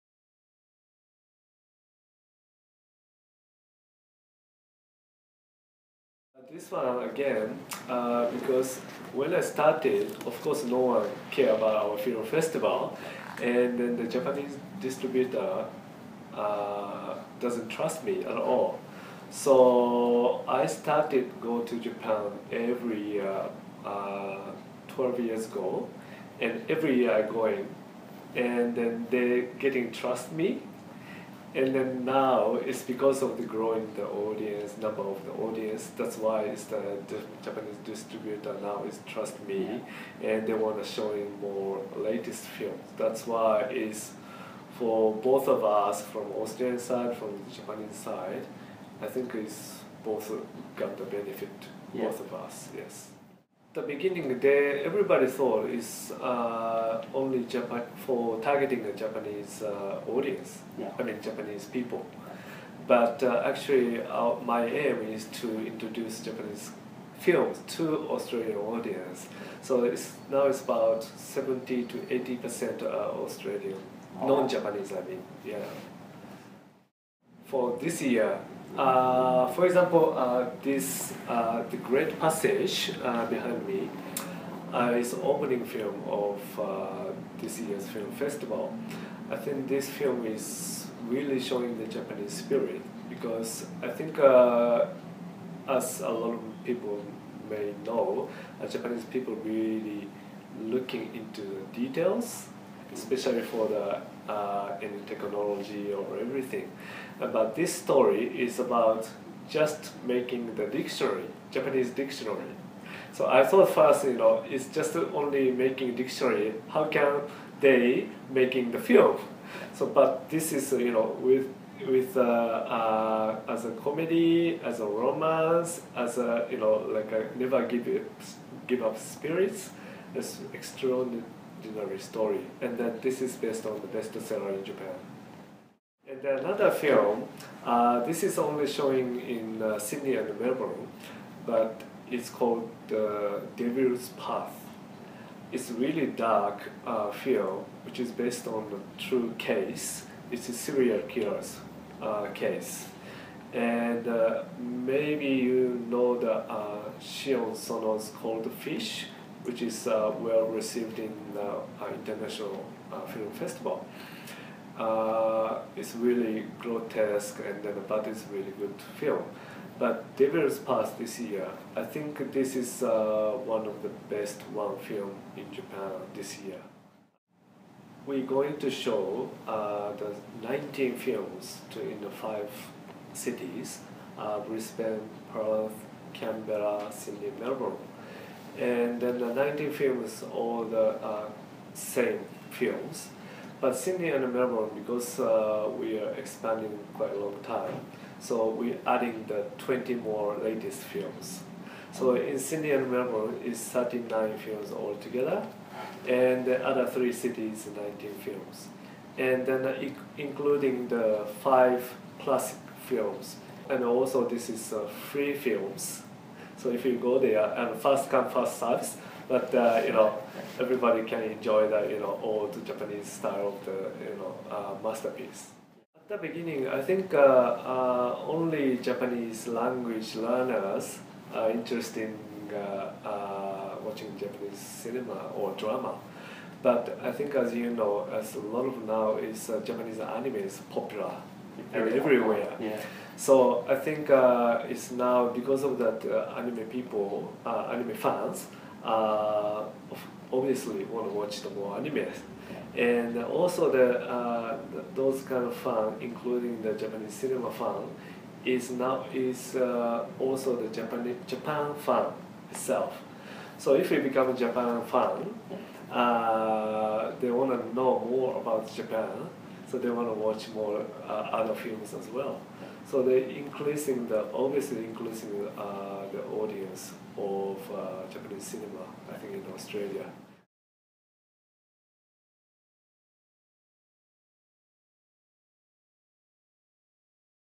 Japanese Film Festival 2013: Mr Masafumi Konomi talks The Great Passage and more
On the eve of the 17th Japanese Film Festival we speak to festival and artistic director Mr Masafumi Konomi. In this second part of our conversation with Mr Konomi he speaks about the interest from Japan and the acceptance of the festival with the Australian audience. He also talks about the opening film The Great Passage and The Devil's Path.

The 17th Japanese Film Festival starts on 13th October in Hobart and runs through till 8th December in Melbourne. The Japanese Film Festival is been shown in most capital cities.

For more information about the screening times and ticketing information for your city

Interview/Camera/Edit: Nathan Atkins

Filmed at the Japan Foundation Office in Sydney, Australia on October 2013